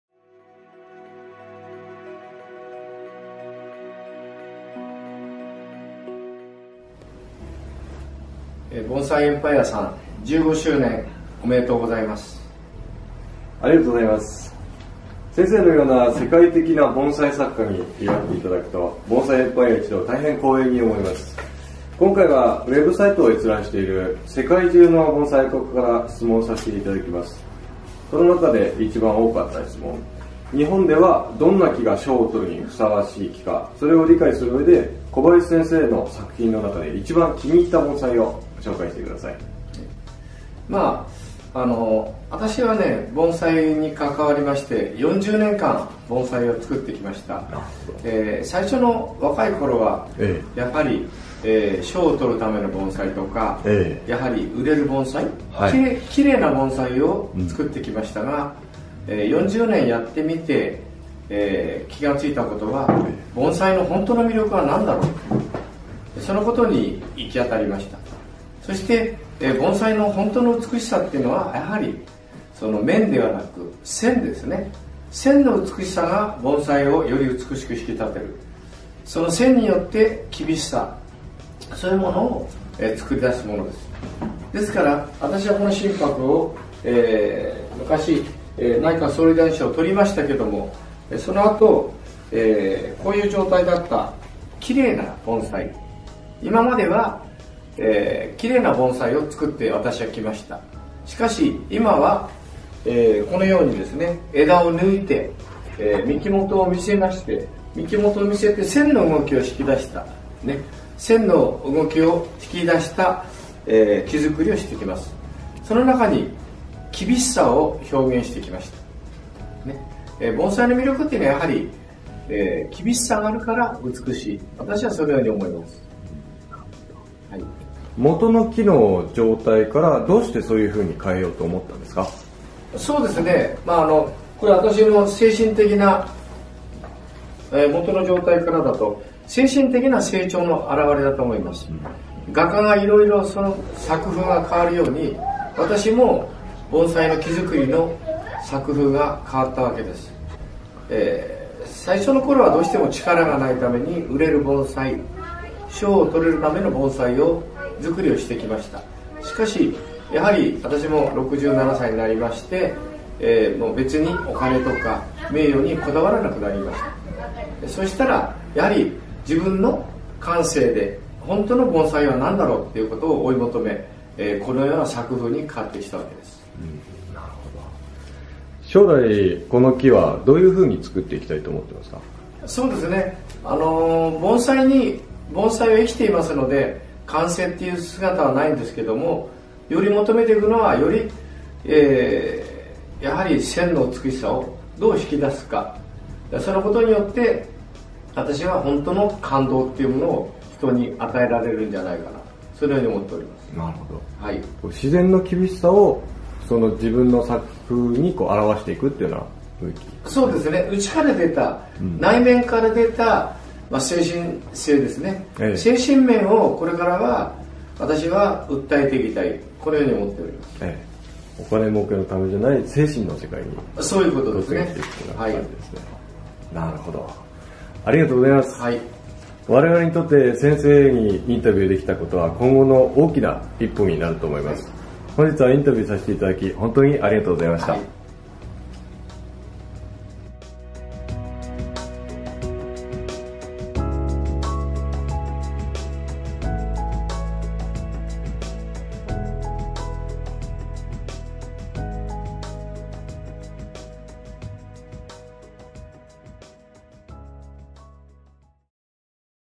What makes a Bonsai truly great? Kunio Kobayashi
Mr. Kobayashi explains what sets a truly beautiful Bonsai apart from others. He talks about his personal style changed over the years, and takes one of his favorite trees as an example of this change.

These movies were made by Bonsai Empire and a panel of international Bonsai experts, to celebrate Bonsai Empire's 15th anniversary.